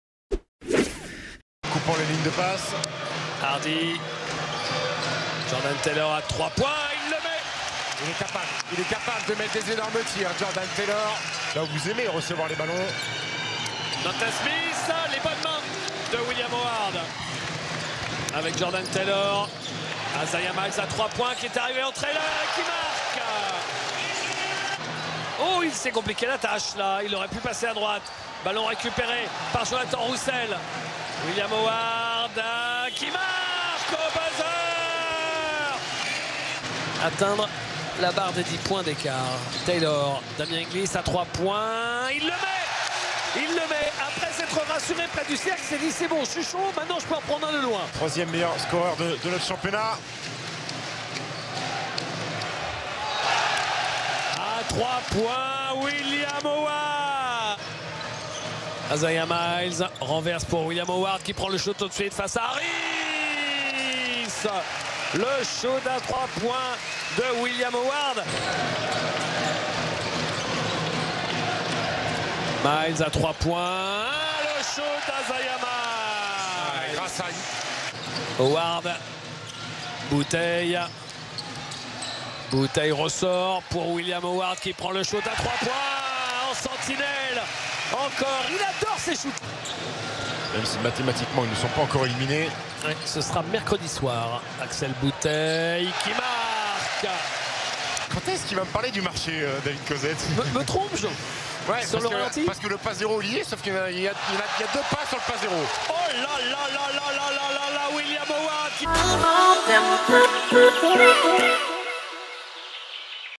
Tous les paniers à 3 pts du CSP lors du classico !
Les 10 paniers à 3 pts du Limoges CSP lors de la belle victoire face à Pau, 81 à 68 !